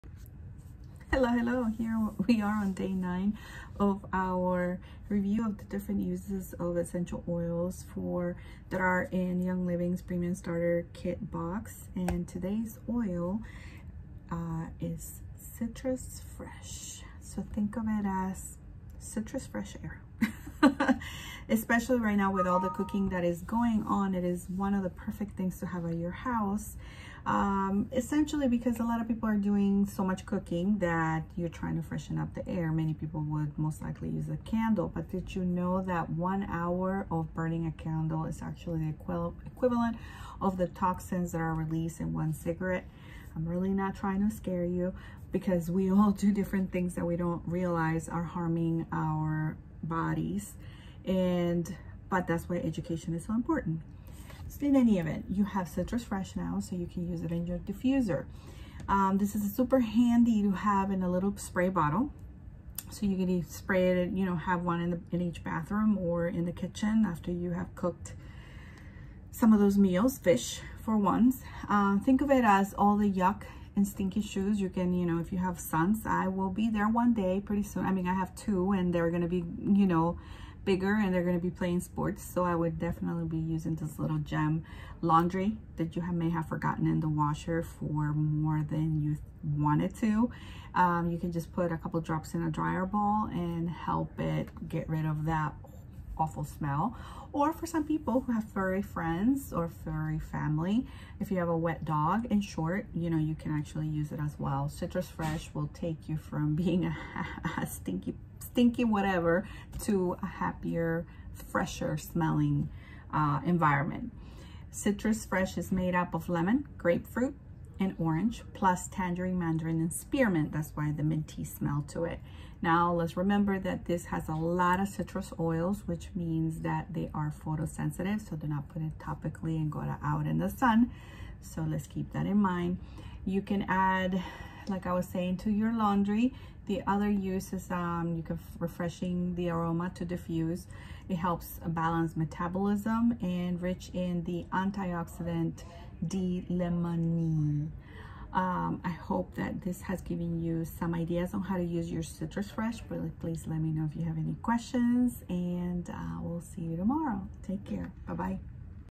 Stay at Home Order ~ Lets learn about Citrus Fresh
Let’s learn about a few uses of Young Living Essential Oils’ Citrus Fresh.